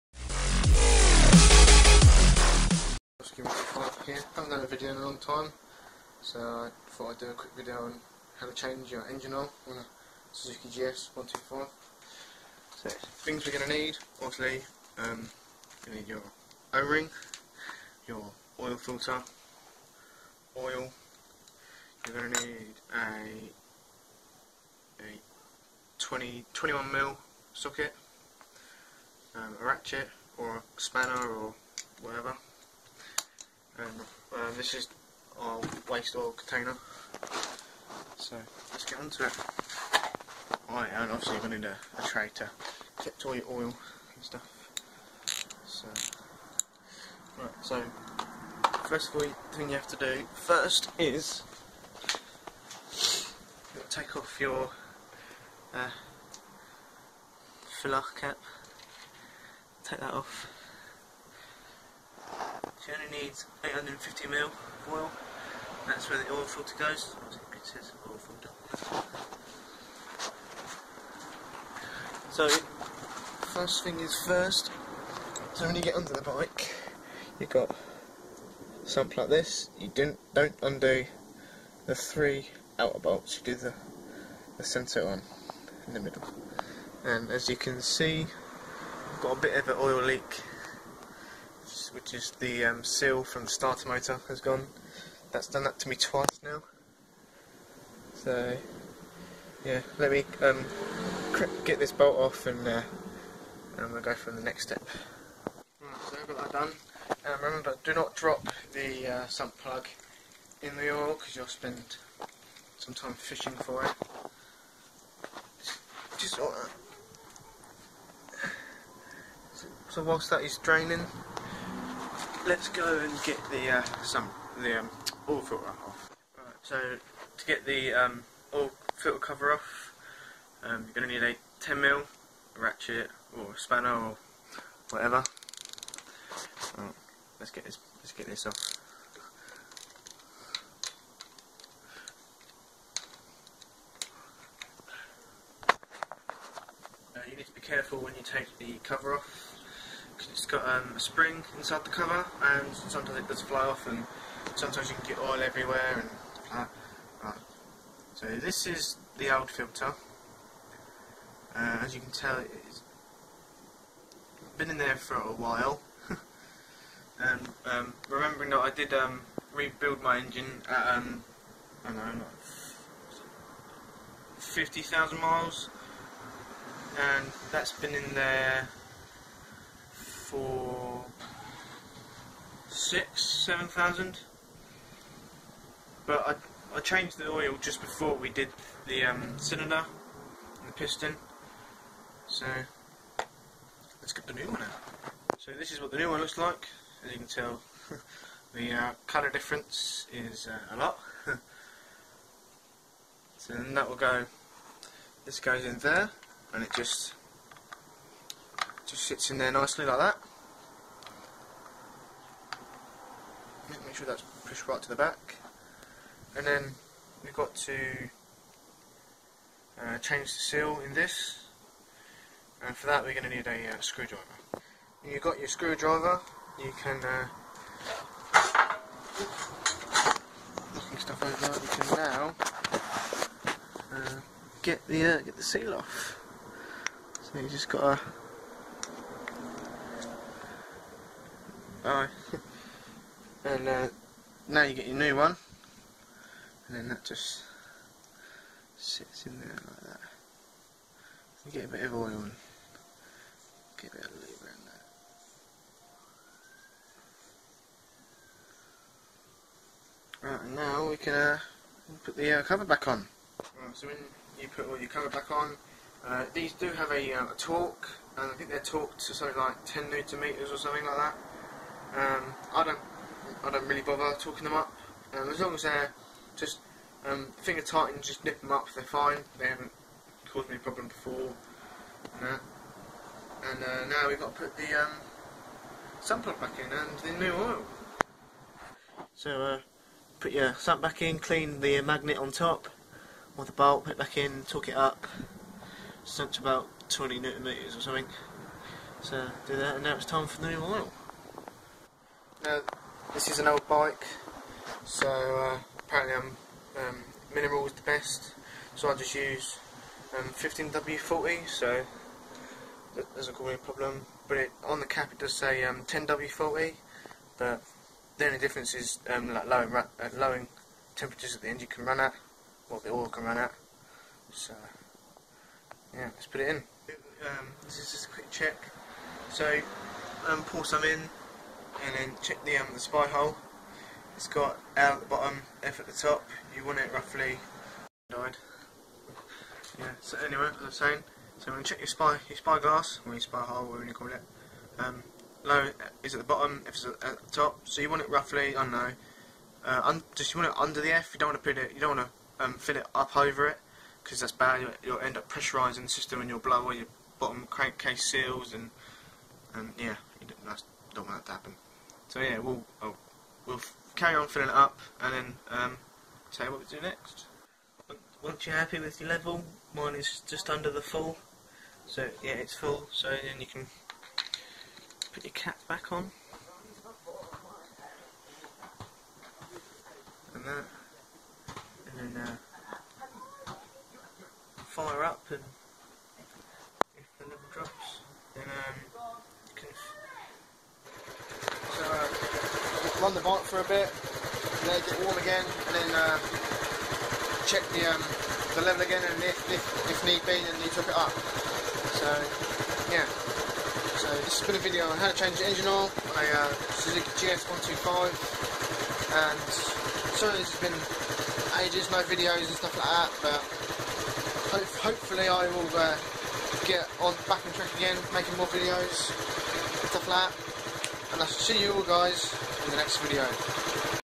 How to change engine oil and filter on a Suzuki gs
Suzuki
Gs 125
How to change
Oil and filter